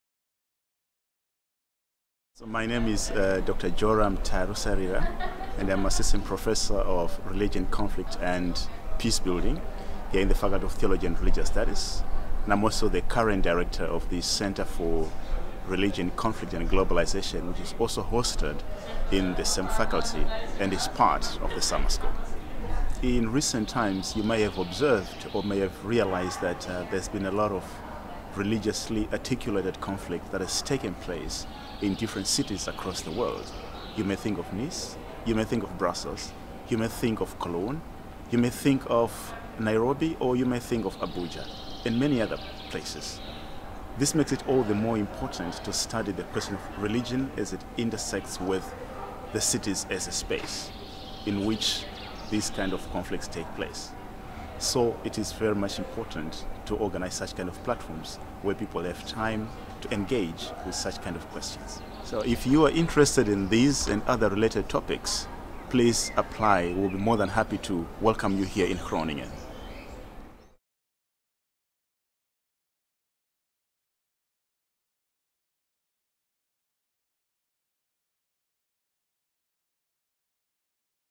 Summer School Religion in Cities 2018: Dr Joram Tarusarira
Summer school University of Groningen about Religion in Cities.
A point of view by Dr Joram Tarusarira.